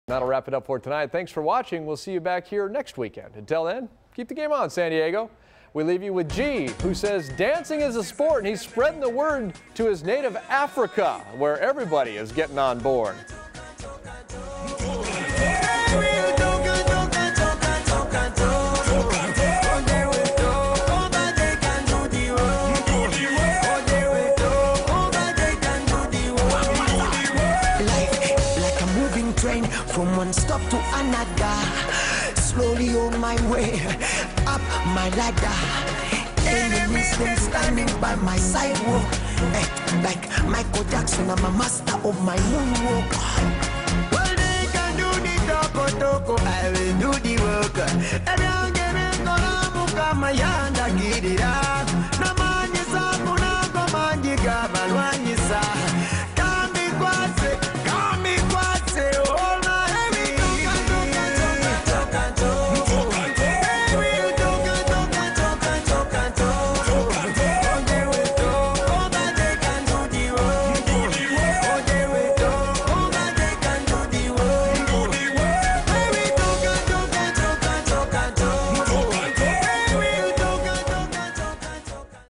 G Dances in Goma, Congo
Got a chance to do a quick "dancing is a sport" segment  with the contemporary twin dancers in the city of Goma, D.R.C.